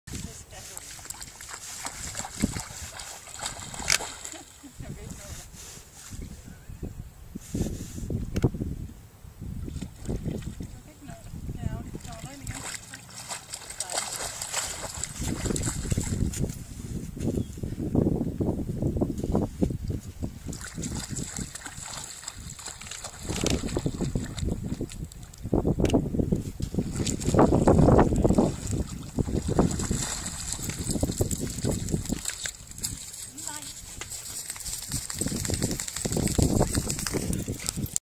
Batsi i Hvide Sande havn
10. april 2011 - Batsi hopper i havnen i Hvide Sande for at svømme med sælerne - vandet er ca 7 grader brrrrrrrrrrrr.